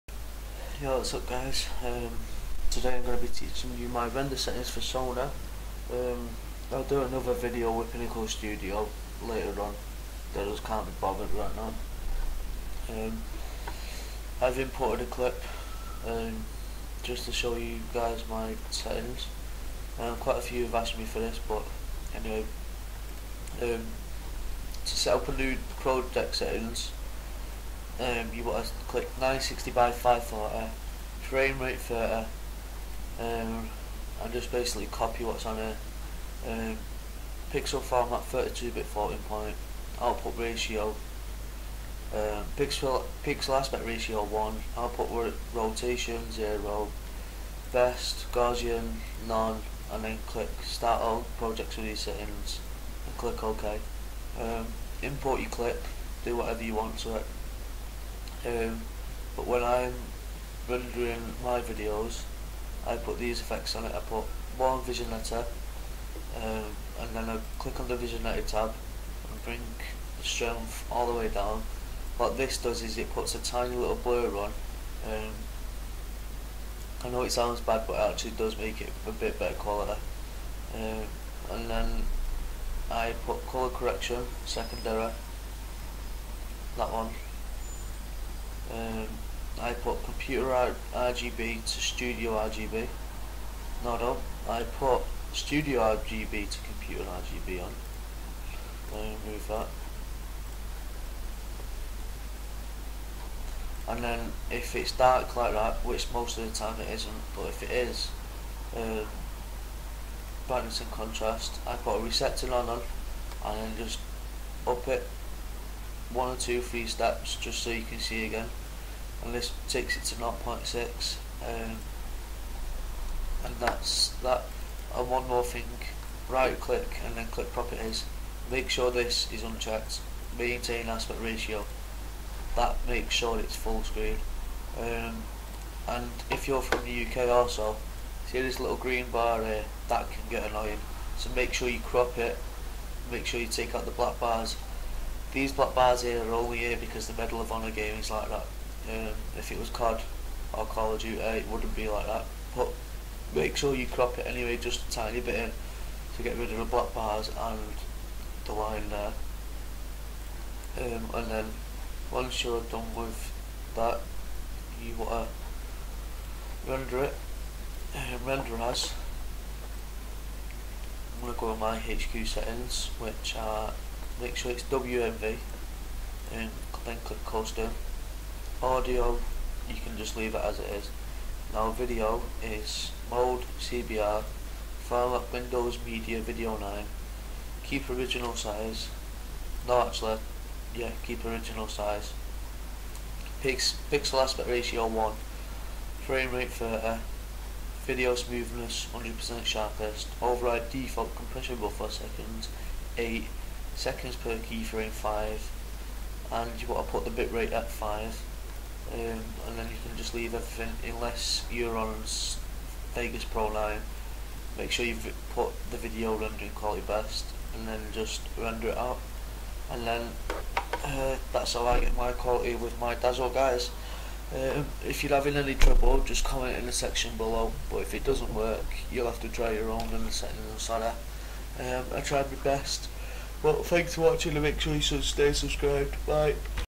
Sony Vegas Quality Settings
This is the exact quality settings I use. I am sorry if they don't work out for you but this is what works best for me :)

I am also going to do a Pinnacle studio settings video as well soon to go with this.

The dazzle I use is the Premium (Black one) 107.